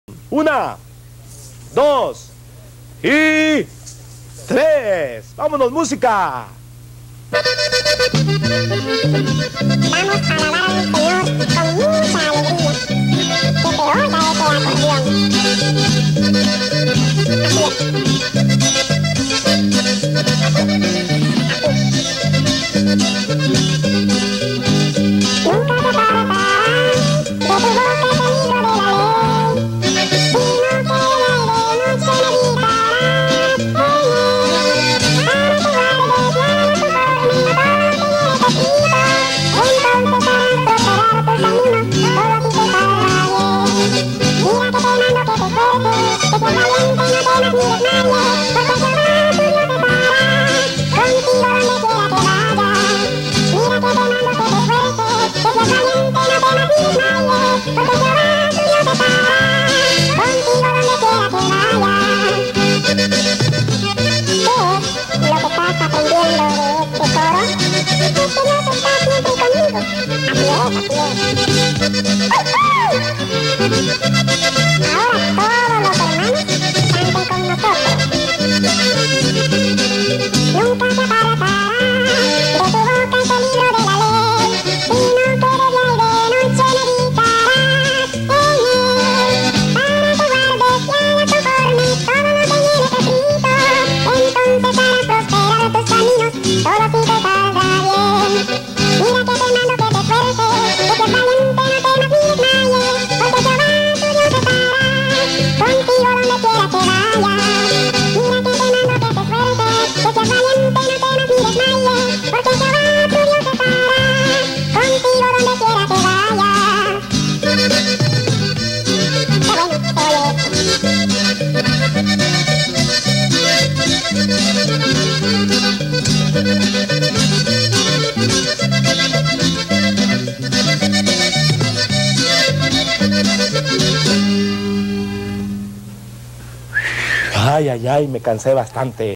The Clown and The Chipmunks
While watching one of the local spanish language television stations, the screen erupted with the clown and the chipmunks.  Television magic.